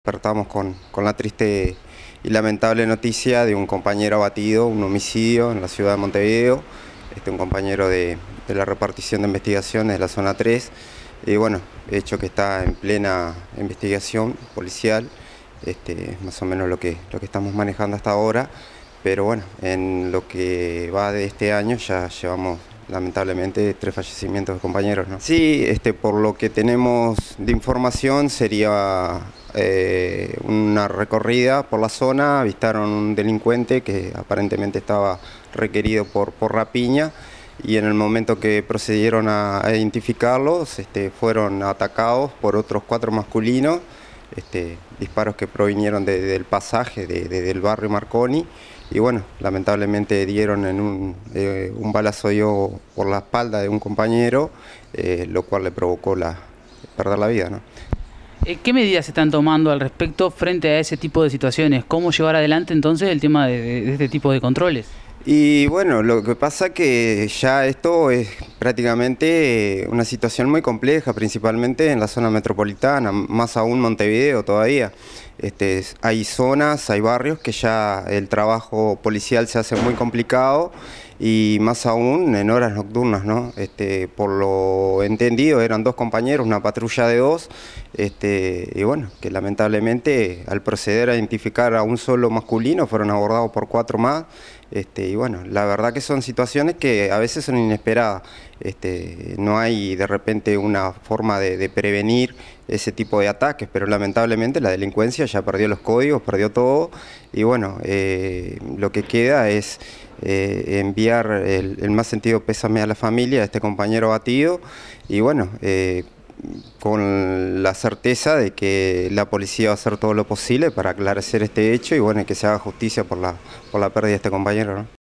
Jorge Clavijo
“Lamentablemente la delincuencia perdió los códigos”, dijo a TV Florida el secretario nacional del SUPU, Jorge Clavijo, al transmitir el pésame a la familia del policía abatido este lunes de madrugada en el barrio Marconi.
“Es una situación muy compleja en la zona Metropolitana, hay barrios donde el trabajo policial se hace muy complicado”, aseguró.
En lo que va del año la familia policial ha lamentado la perdida de tres efectivos, dos por suicidio en enero y éste a mano de la delincuencia.